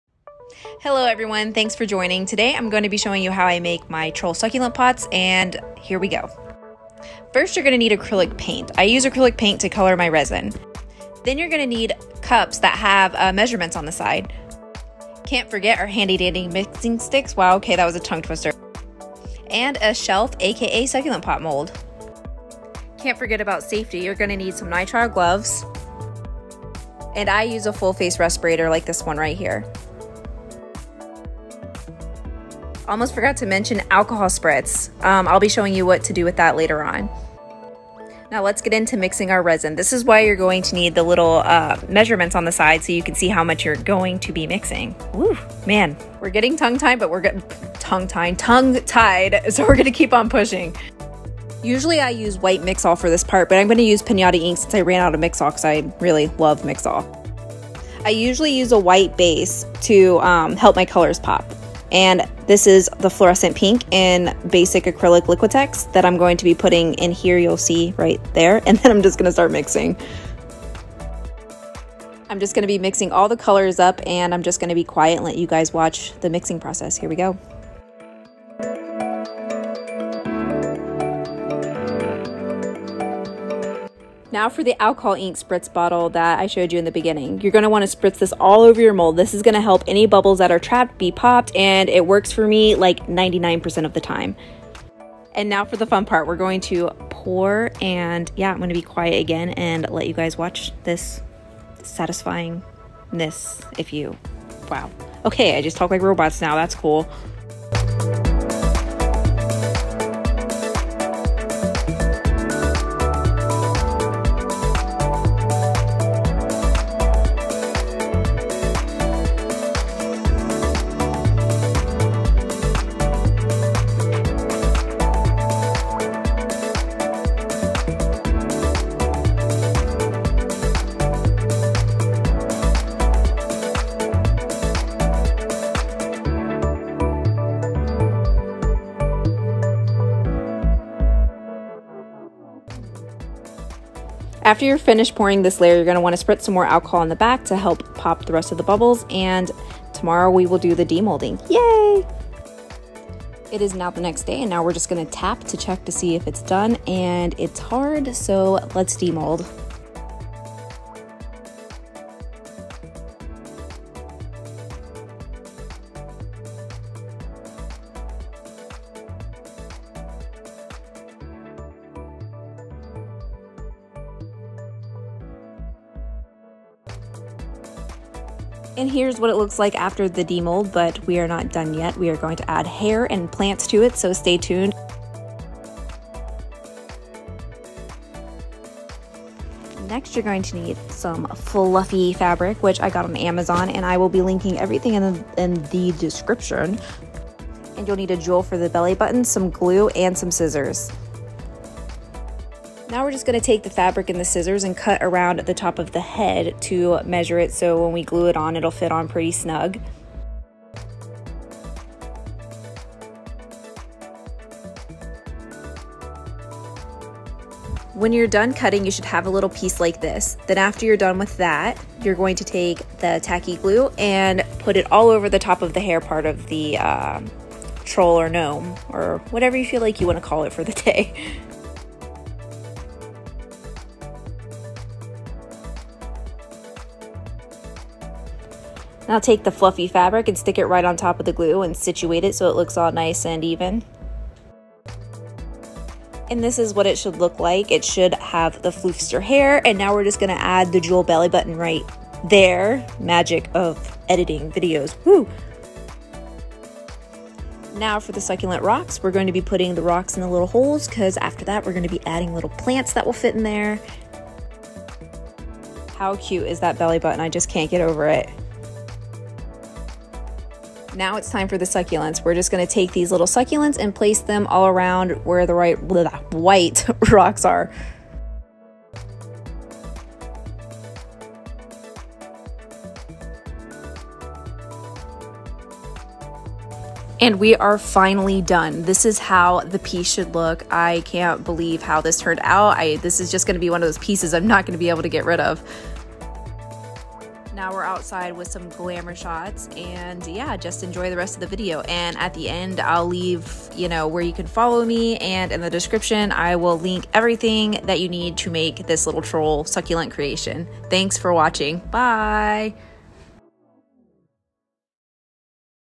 How to make succulent pots using Biskuit’s shelf molds
Where do you get your products?

Molds: biskuitsandgravy.Etsy.com

Paint: Michaels Craft Store- Liquitex Paints

Fluffy fabric: Amazon- YYCRAFT Luxury Thick Heavy Faux...

Gems for gnome belly button: KraftGenius Allstarco 11mm Flat...

Pro Tip: USE Mas Epoxy Deep Resin to do deeper pours. I ran out so, I used Table top. If you use table top resin YOU MUST POUR IN LAYERS! If you don't it'll cause and exothermic reaction and cause bubbles which you can see in the hair of the troll. Table top is perfect for glossing and thin pours but not deep pours all at once. That didn't really matter to me in this creation because I knew I would be covering it with fabric. Use this link to get their deep pour resin here.